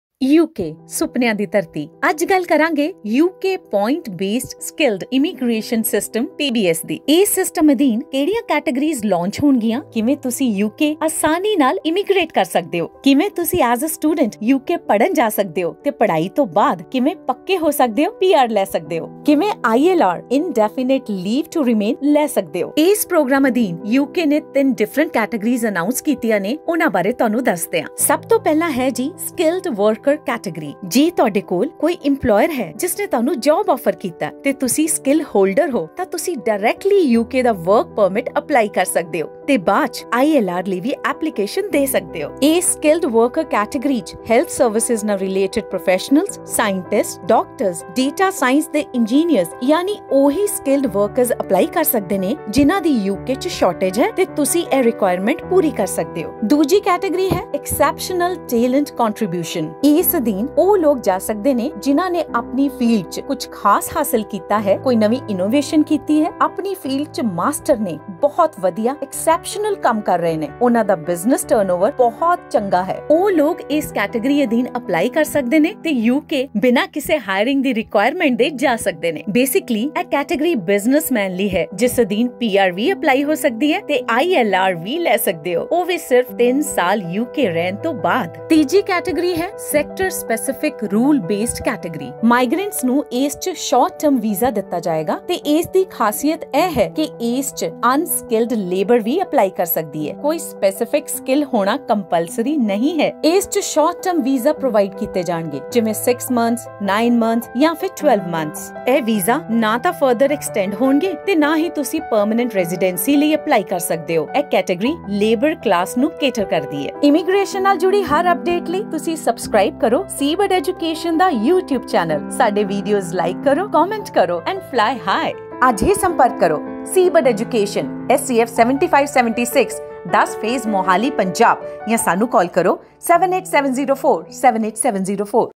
How to get PR in UK ? (PBS System)
UK is launching Point Based Skilled Immigration System (PBS).

Apply for STUDYVISA for UK and get PR & ILR in future.

Different Categories:
* Skilled Worker Category.
* Exceptional Talent Contribution.
* Sector Specific Rule Based Category.

Apply Now For 2020 Intake...